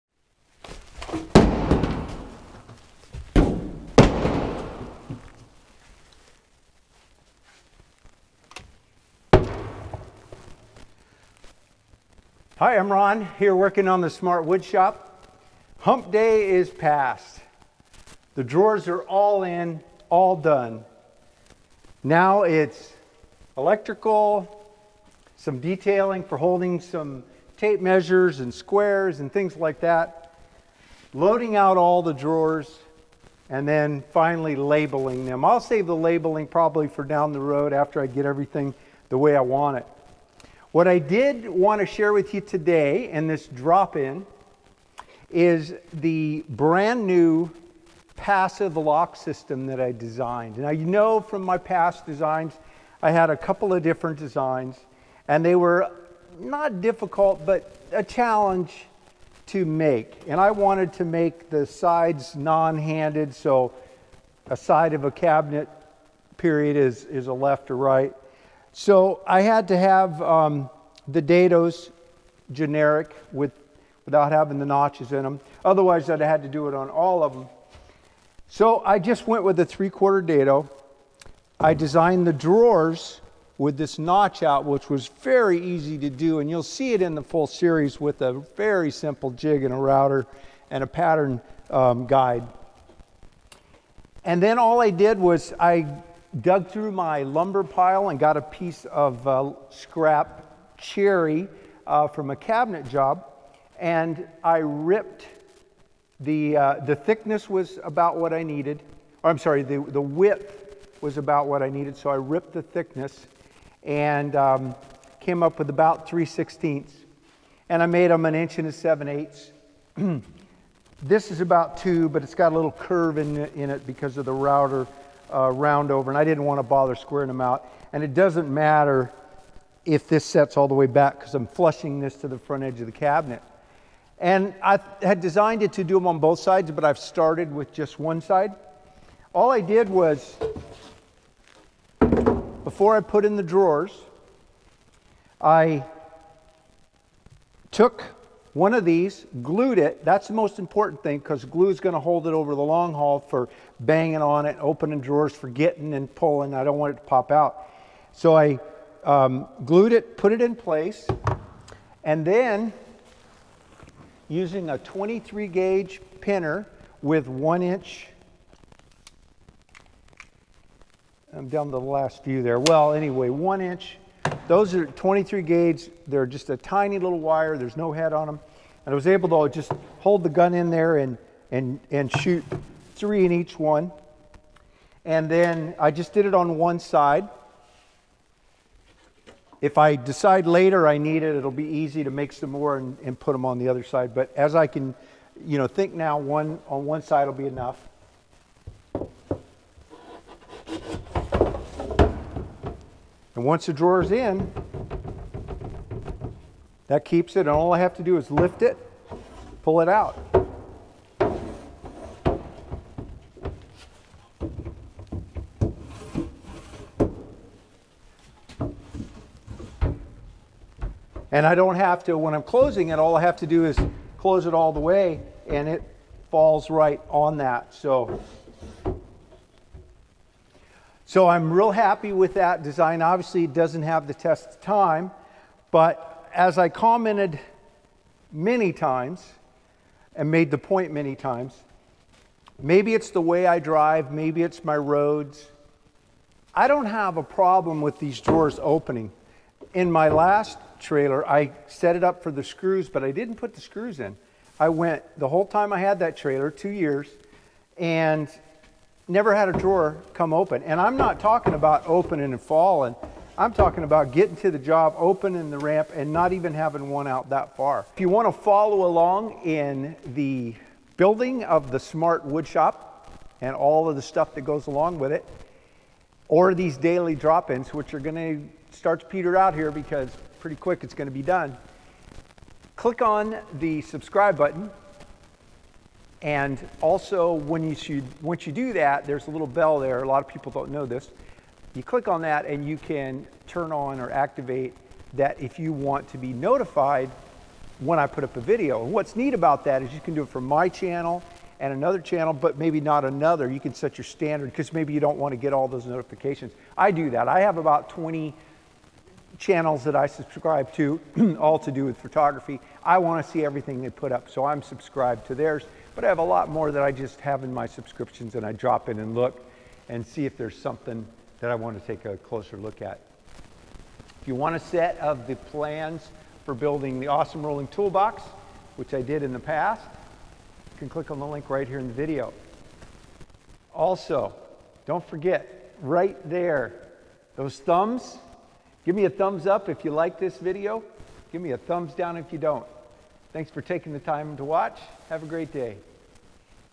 YES!!!! It Works: Free easy passive drawer lock!!!!!!!!!
The passive drawer locks are done and free to make.

Aftershokz Headphones
coliid=IJD9PZJRC2MJY&colid=1IZTLADR6FX0Z